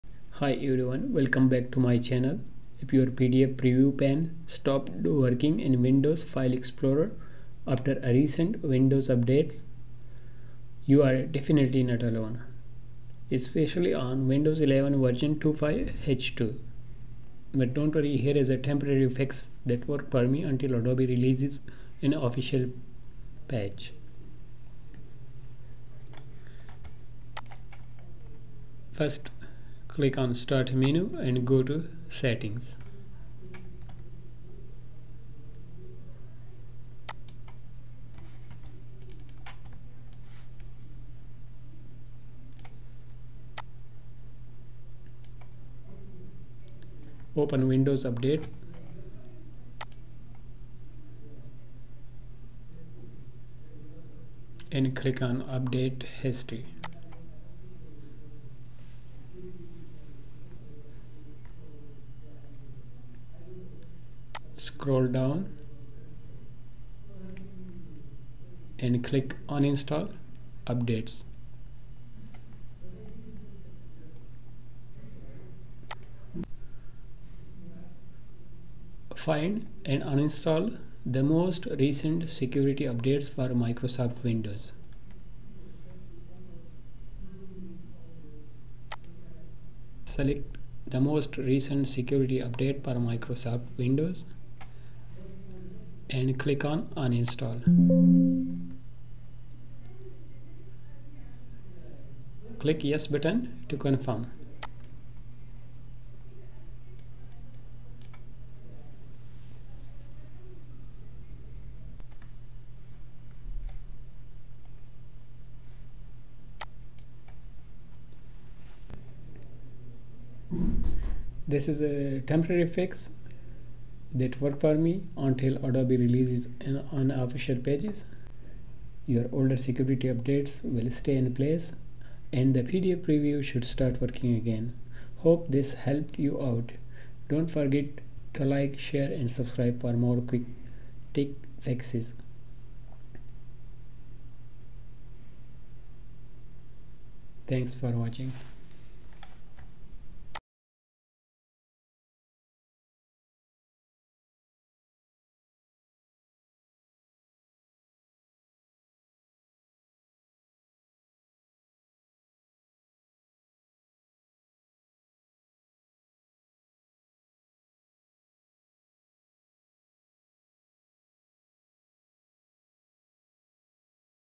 PDF Preview Not Working After Windows Update | Fix PDF Preview Pane in Windows 11 File Explorer
If your PDF Preview Pane stopped working after a recent Windows update, you’re not alone! Many Windows 11 users (especially on version 25H2) are facing this issue where PDF files no longer show previews in File Explorer. In this video, I’ll show you a simple and effective workaround to bring back your PDF preview pane — especially useful if you use Adobe Acrobat or Reader.

⚙️ What’s Covered:
Why PDF preview stopped working after update
How to fix or re-enable PDF preview in File Explorer
The file you are attempting to preview could harm your computer

💡 Works with:
Windows 10 | Windows 11 | Adobe Acrobat | Adobe Reader